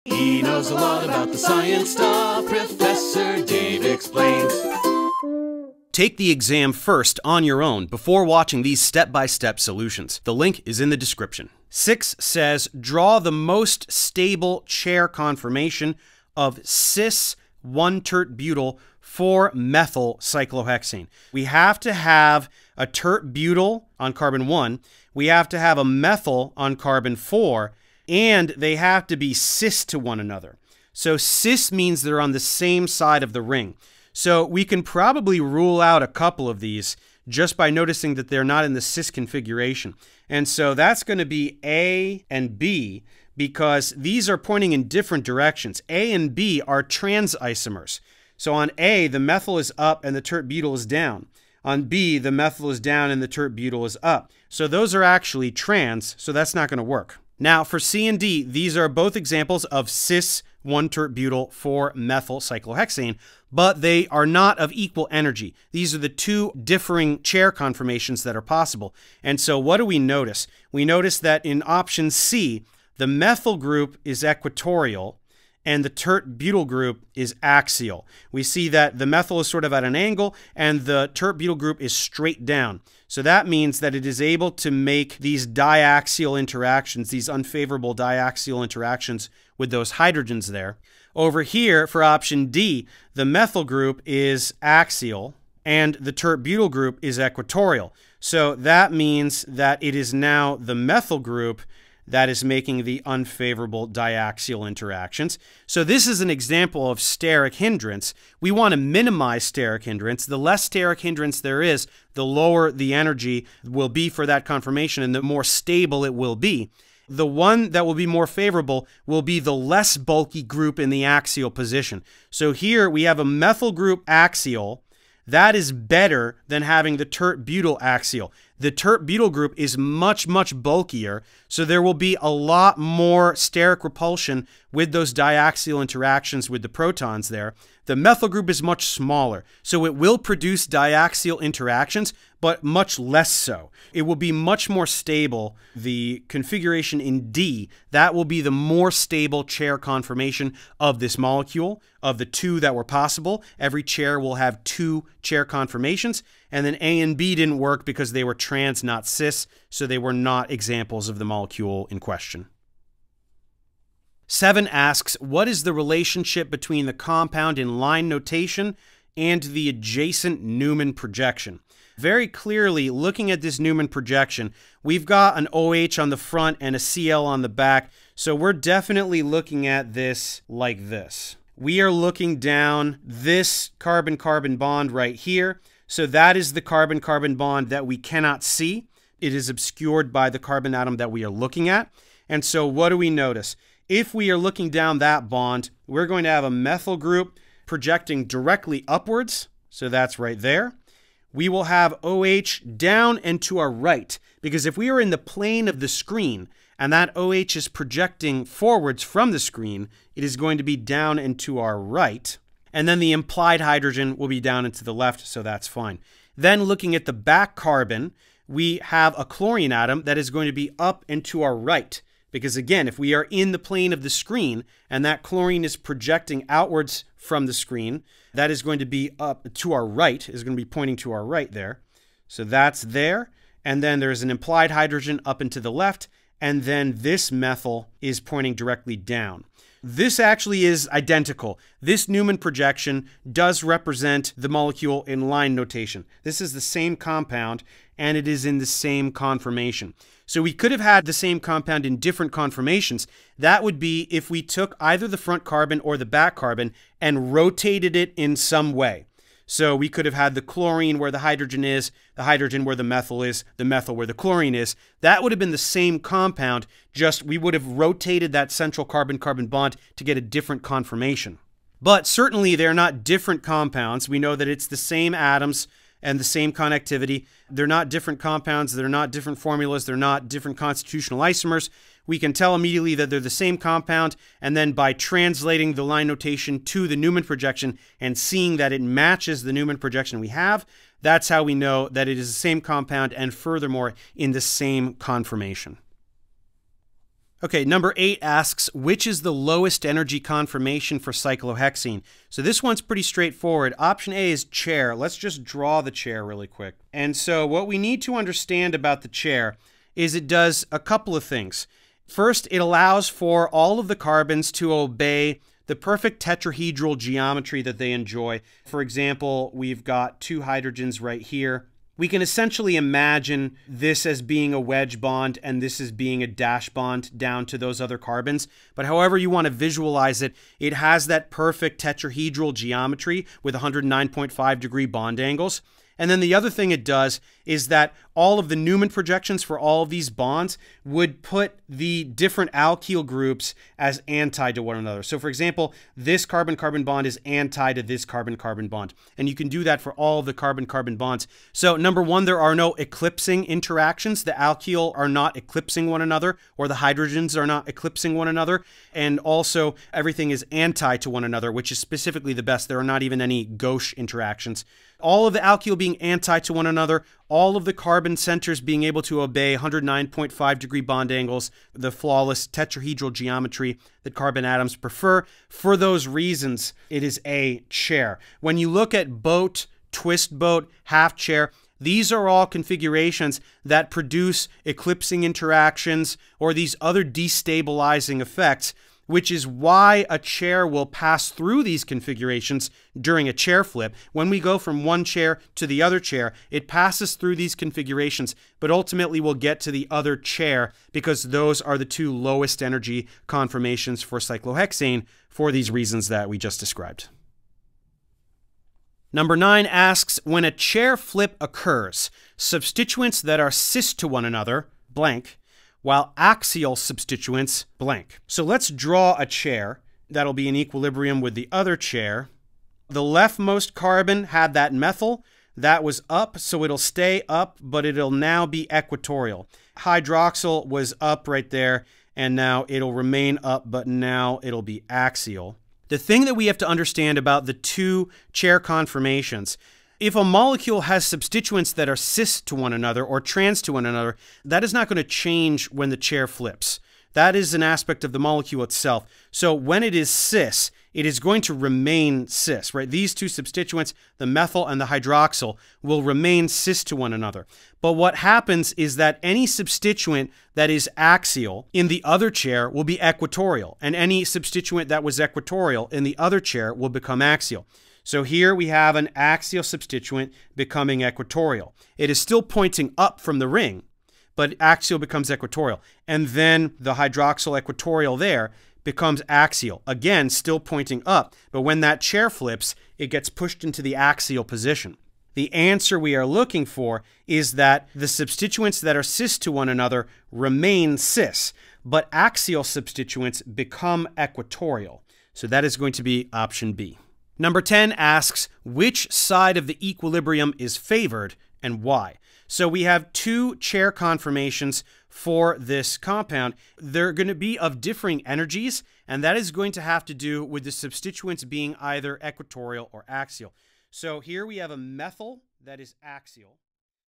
Would You Pass My Alkanes and Cycloalkanes Organic Chemistry Exam?
Make sure to take my Alkanes and Cycloalkanes Practice Exam here first

This practice exam will test your comprehension on Newman projections, chair conformations, chair flips, constitutional isomers, and more!